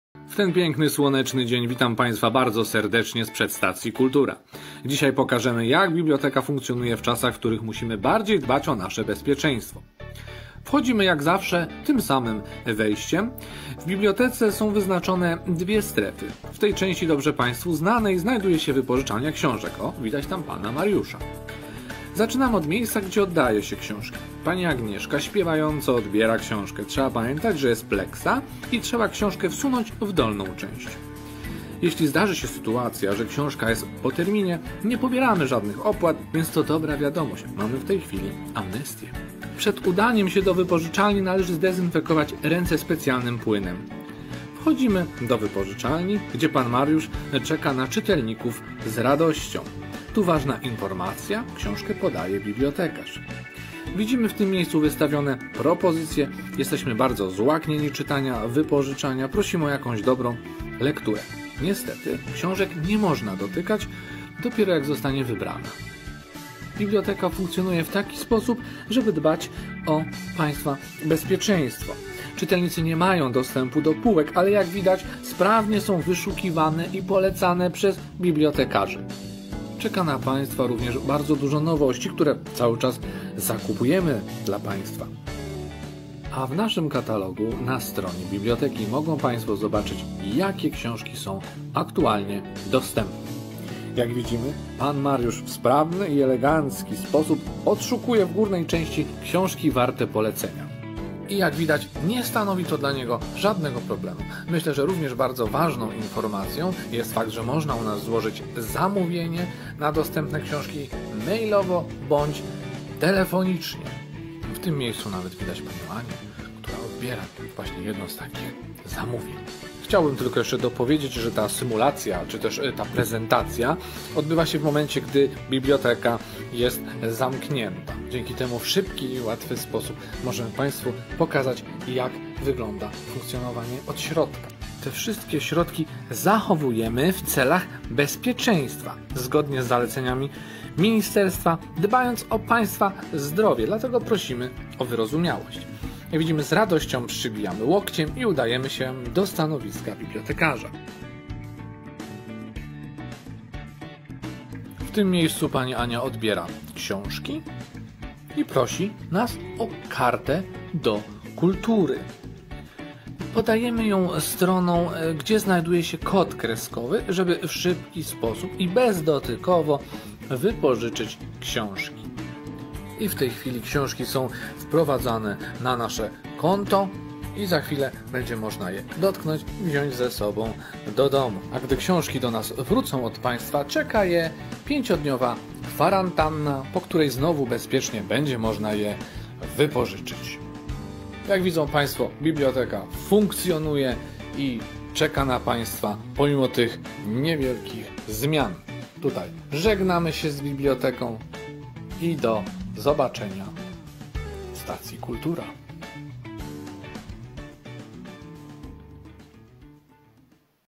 Stacja Kultura w czasie pandemii 2020 feat. pan Mariusz
🔊 Stacja Kultura w czasie pandemii 2020 - instrukcja obsługi biblioteki
👏 feat. pan Mariusz [włączcie dźwięk!]
👏 zdjęcia - pani Ania
❓ Boicie się przyjść do biblioteki po ponownym otwarciu? Może martwicie się, że strzelicie gafę? ▶️ Obejrzyjcie filmik, a wszystko będzie jasne!
❓ A może byliście już w bibliotece po ponownym otwarciu? Sprawdźcie, czy robiliście to dobrze.▶️ Niech kojący głos naszego kolegi Was poprowadzi...